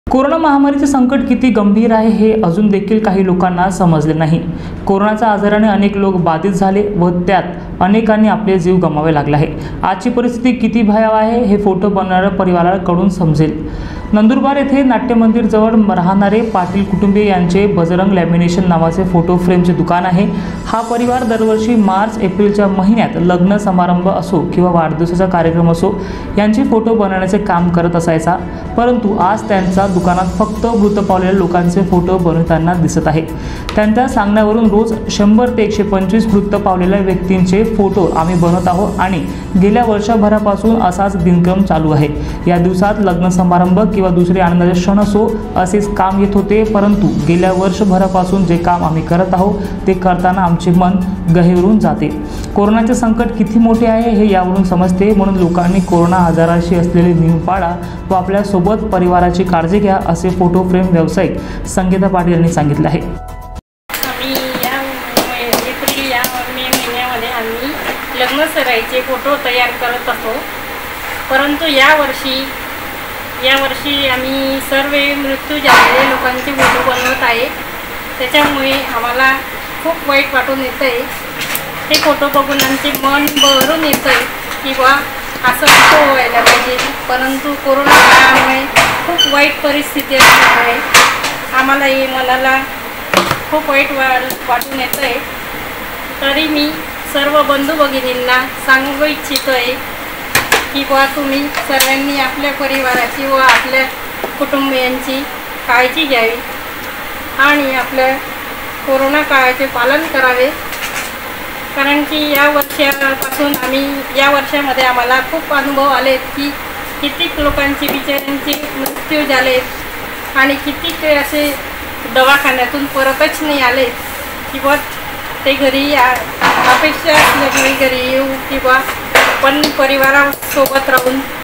दिवसाला 100 ते 125 मृत व्यक्तिचे फ़ोटो बंनविताना मन गहीवरते | 25 APRIL 2021
परिस्थिति किती भयावह आहे हे  फ़ोटो बनवणाऱ्या परिवारा कडून समजेल | 25 APRIL 2021